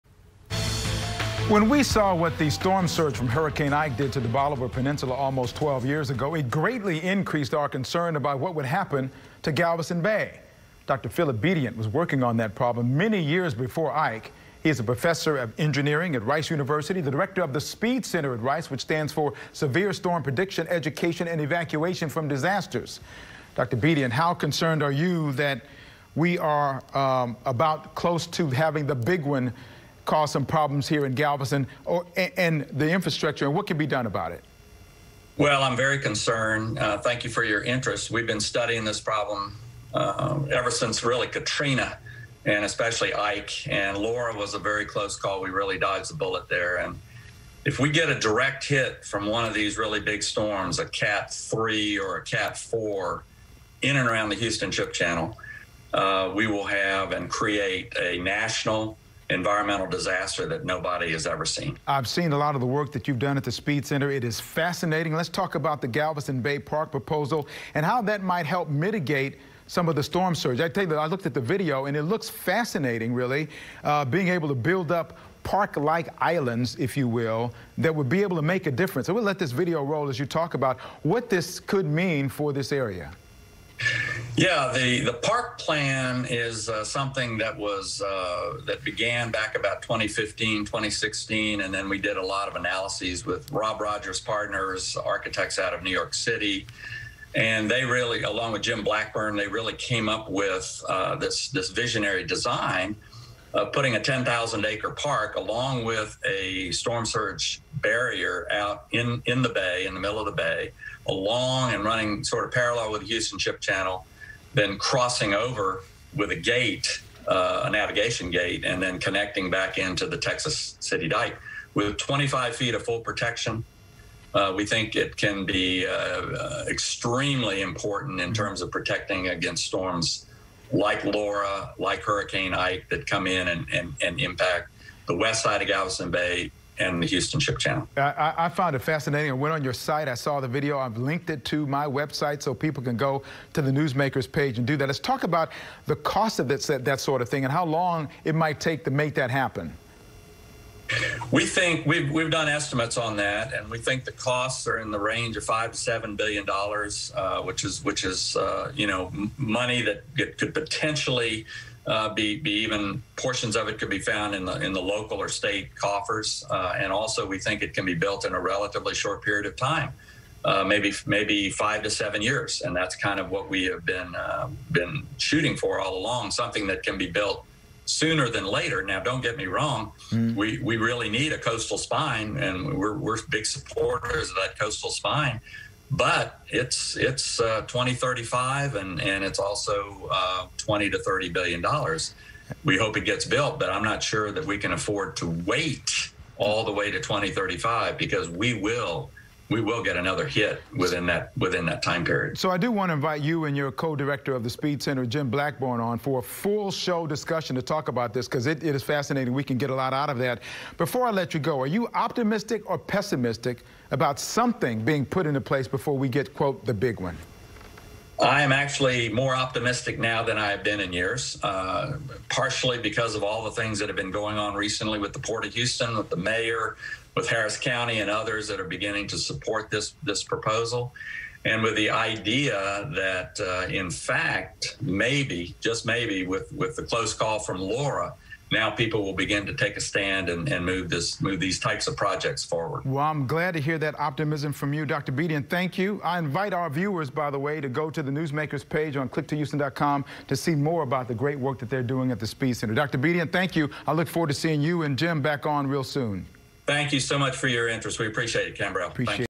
Galveston Bay Park: Can a Five Billion Dollar plan work?
Rice engineering professor Dr. Philip Bedient is the director of the SSPEED Center (Severe Storm Prediction, Education and Evacuation from Disasters).He is a guest this week and talks about the proposal to build a series of islands in Galveston Bay that would work to mitigate a higher than normal storm surge that might come with a hurricane.